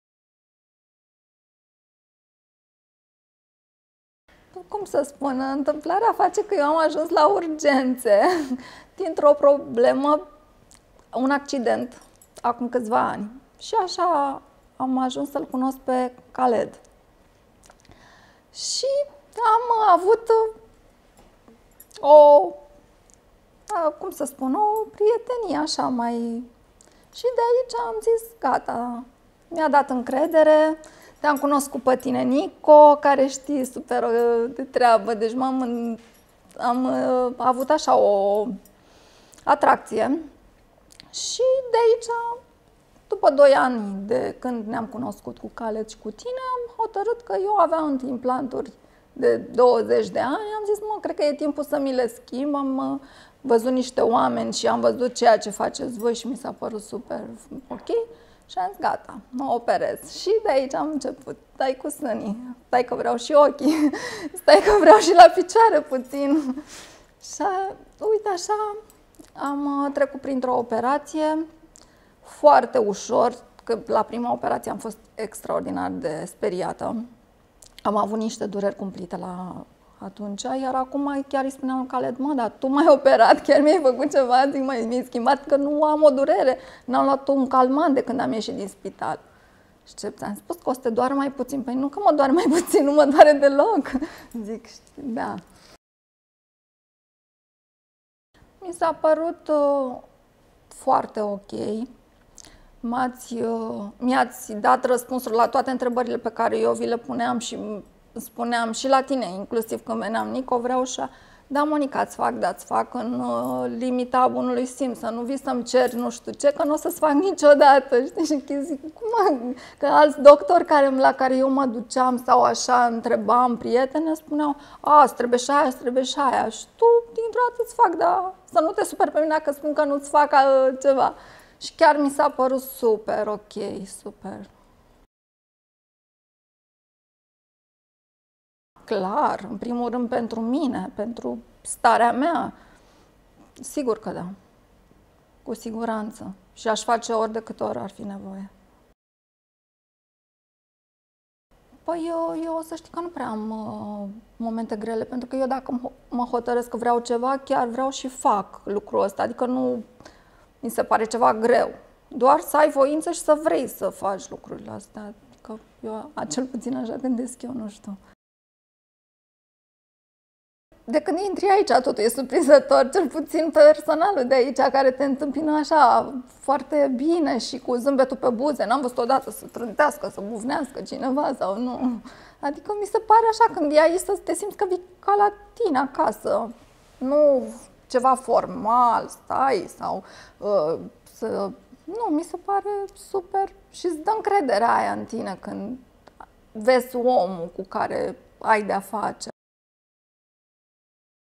Testimonial chirurgie estetica @ Soma Clinic Bucuresti
Experienta personala a fiecarui pacient conteaza  pentru echipa noastra foarte mult. De aceea ne-am gandit sa va punem la dispozitie pe canalul nostru o serie de testimoniale video cu cateva dintre pacientele noastre, primele care au fost mai curajoase.
Ne-am gandit ca nu este atat de important ce fel de operatie estetica are fiecare pacienta, deoarece intimitatea fiecaruia trebuie respectata cu rigurozitate. Mai important este sa vezi cum a decurs intregul proces si cum a influentat increderea in propria persoana si stima de sine o operatie chirurgicala estetica la Soma Clinic.